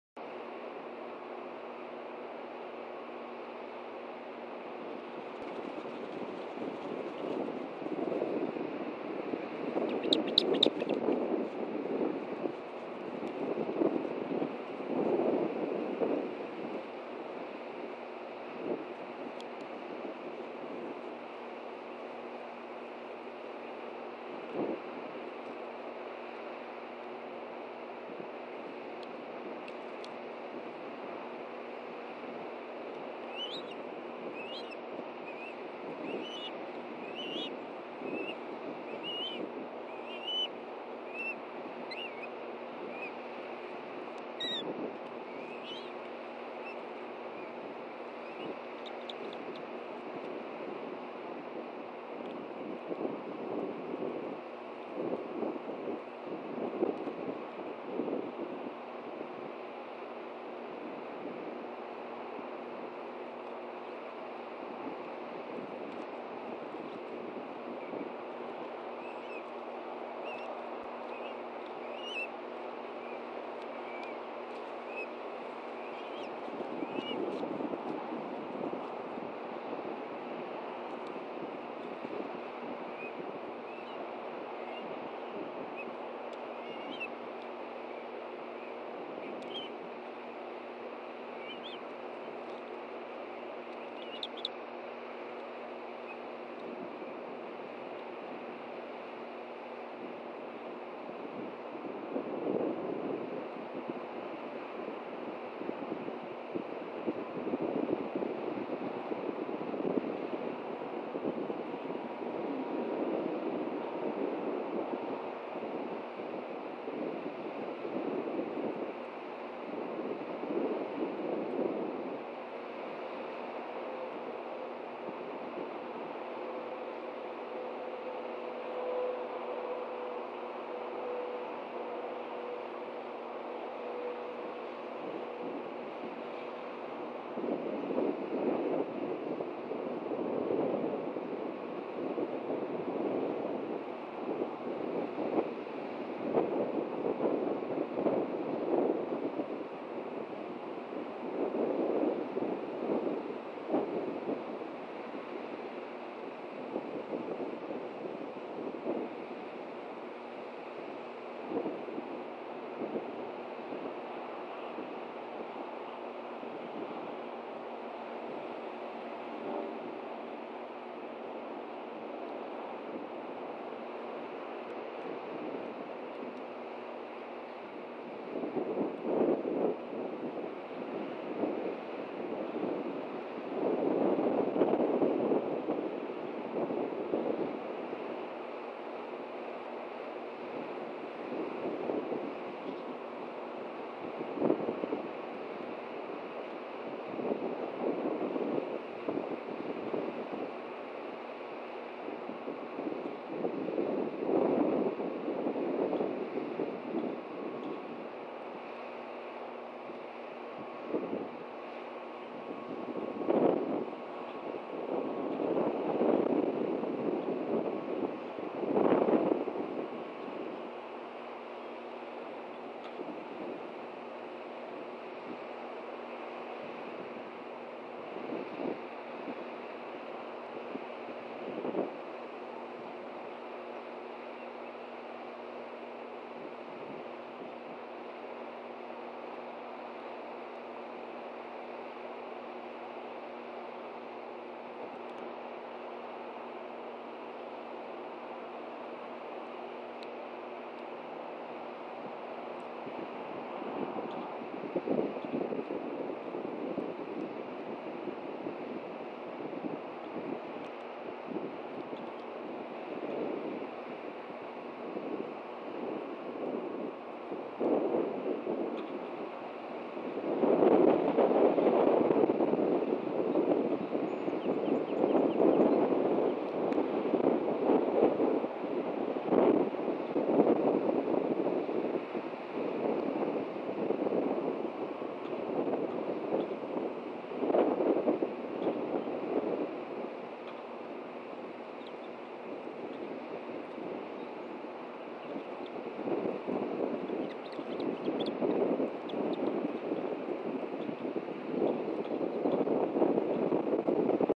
Video 2013 07 11 231247 ~ THE STICK THAT CAUSED A FIGHT!
Project Osprey, July 11, 2013.  Iris brings in a stick, pins down Taylor!  Taylor gets free and takes out the pain on Miles.  Miles gets down and Iris decides to move the stick again causing Miles to stumble out of the mess of sticks.